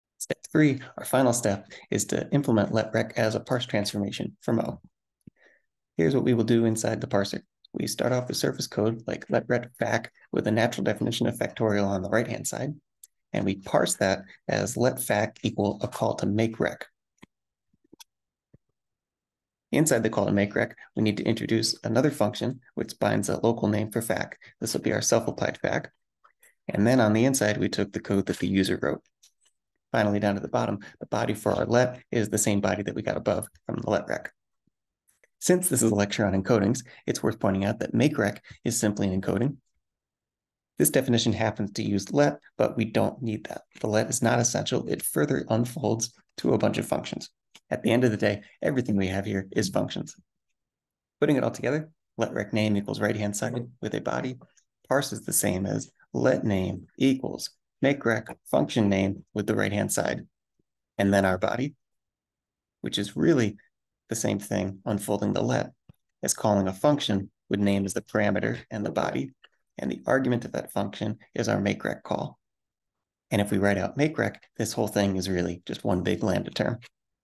Shplait Y 4 - parsing letrec
Our third and final step in understanding the letrec encoding is to identify the equivalent expression that parse can produce for an input letrec form.